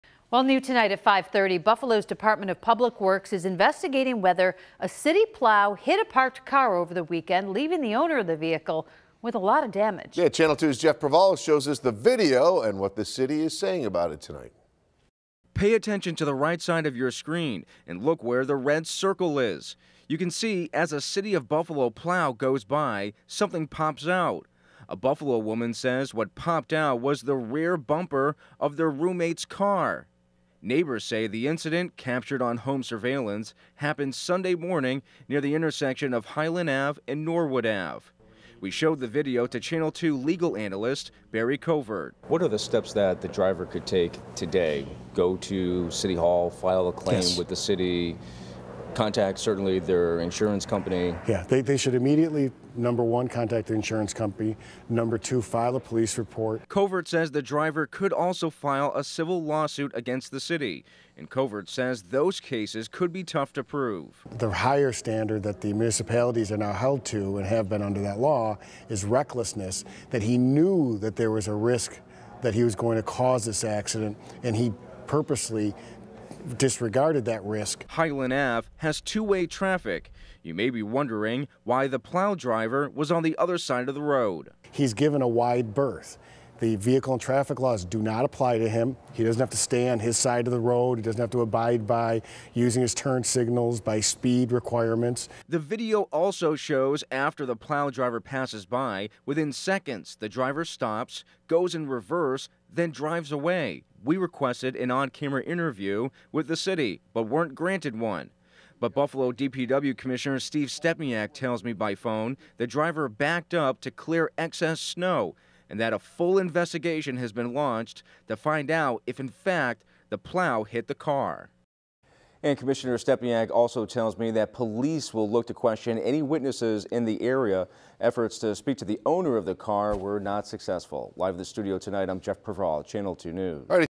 City Plow Hits Car
Buffalo Investigating Whether Plow Struck Car